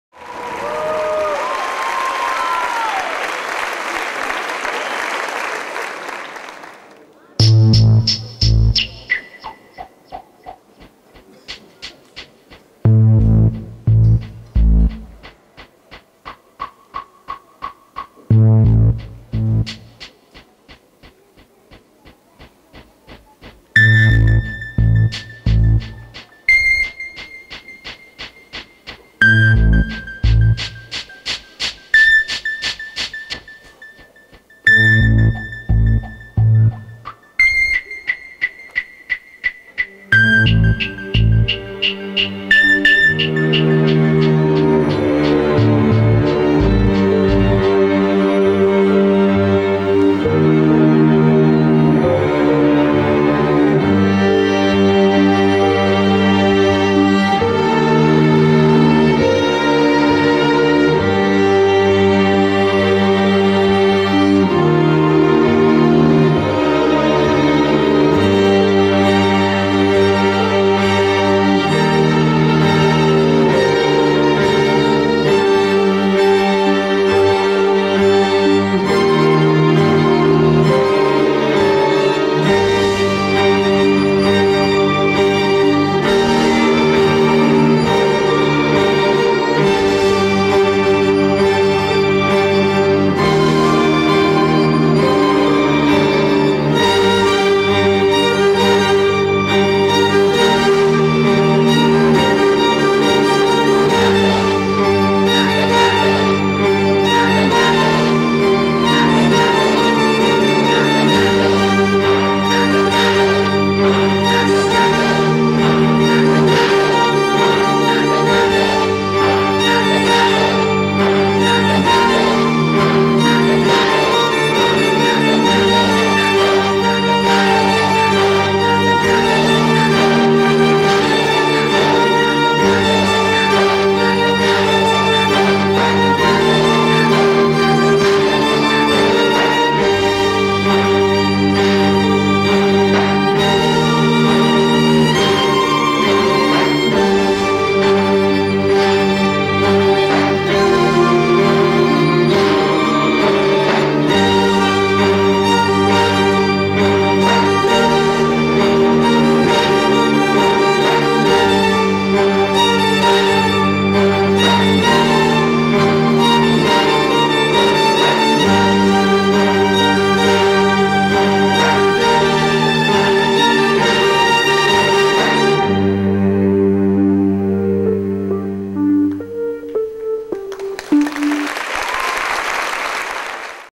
AS TRATTO
Balletto del "Saggio di Fine Anno" del 2012.
Coreografia di Antimo Lomonaco